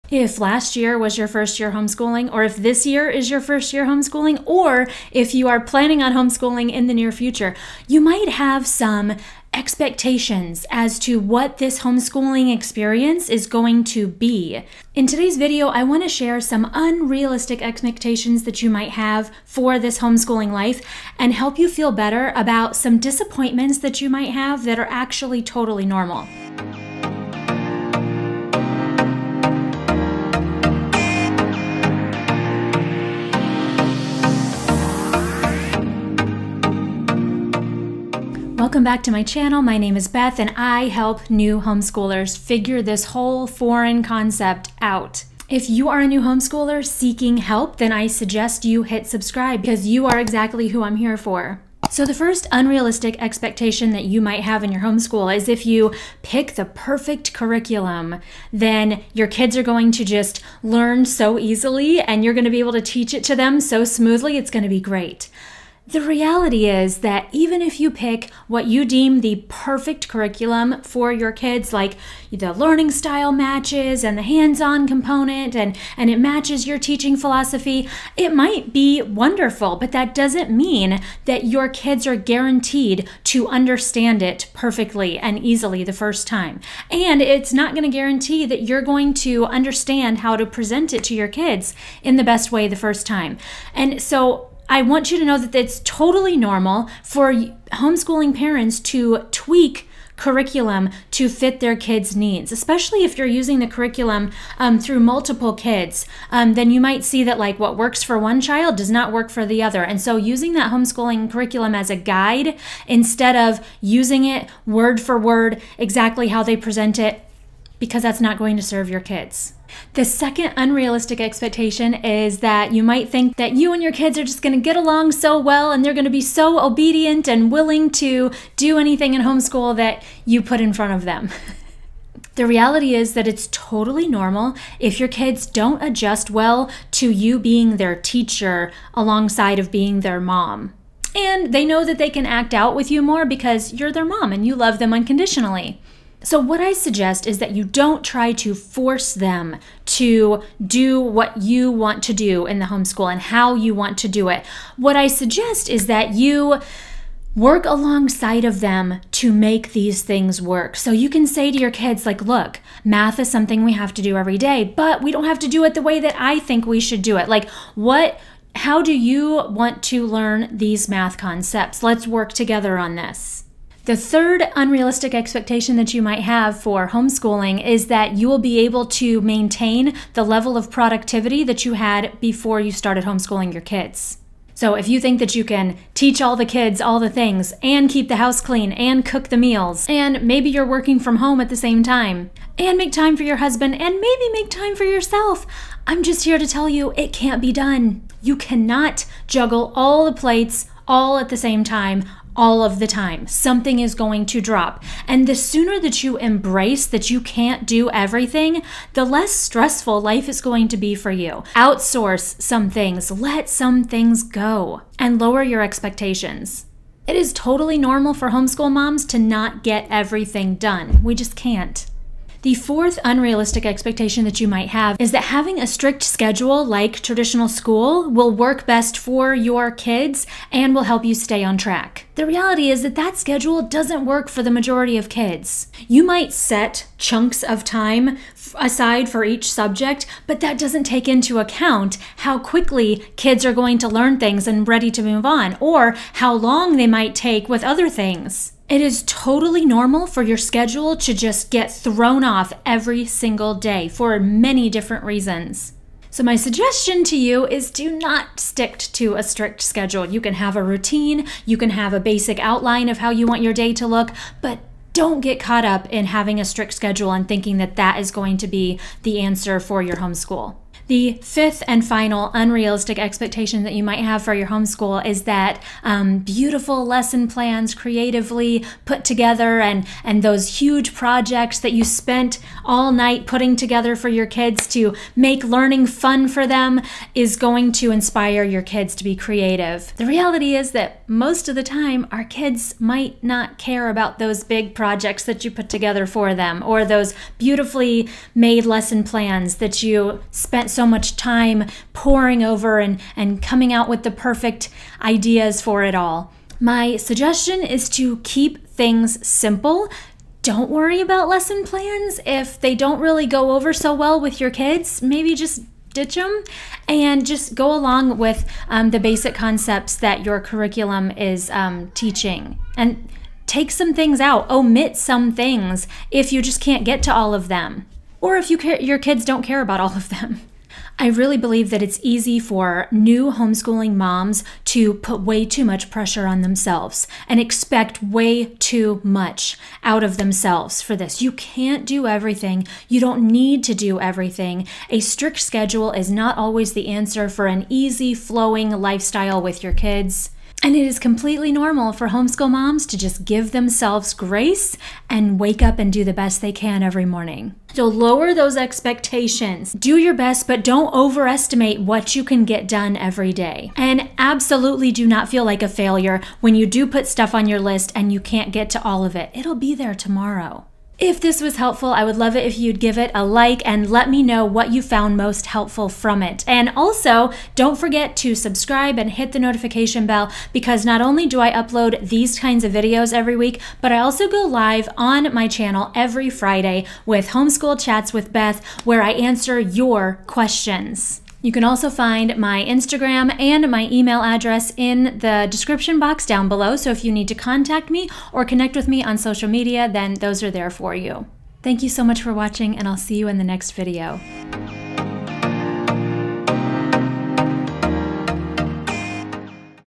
Homeschool For Beginners: 5 Unrealistic Expectations to Avoid
If you are a new homeschooler, there are 5 unrealistic expectations you should avoid.
In today’s video I want to share some unrealistic expectations you might have for your homeschooling life, and help you feel better about disappointments that are actually totally normal
1.  If I pick the perfect curriculum, my kids will easily understand and I will easily be able to teach them.
a. Reality: that perfect curriculum might not work perfectly for your kids, or it might work for one kid but not the other. It’s totally normal for kids to need you to adjust the curriculum to fit their needs.
2. My kids and I will always get along and they will be so obedient and agreeable when homeschooling.
a. Reality: It’s totally normal if your kids don’t adjust well to you being their teacher. And they know they can act out more with you because you’re their mom. I suggest not trying to force them to do things the way you planned them, but let them know that this is what you are going to teach them and they can help structure how you do it.
3. I’ll be able to get all work done with my kids, keep the house clean, cook all the meals, work for my job from home, make time for my husband, and also make time for myself.
a. Reality: you can’t do all the things all the time. The productivity level you had before you homeschooled your kids can’t be met anymore. Outsource some things, let some things go, lower your expectations. You are not supermom, and it’s totally normal for homeschool moms to not get everything done.
4. Having a strict schedule like school will work best for us to stay on track.
a. Reality: That schedule doesn’t work for the majority of kids; you might set blocks of time aside for each subject, but that’s not accounting for the kids taking longer with some concepts, or getting a concept really quickly before the time block is up.
5. Your perfectly-created lesson plans or schedule that you thought would be fun and engaging for your kids will go over beautifully.
a. Reality: The kids might not like your lesson plans or fun activities that you create for them. It is normal for things to not go as you planned them. I highly recommend you just roll with it.

My mission at Teach From Home is to help new homeschoolers get started with everything they need to confidently and happily homeschool their kids.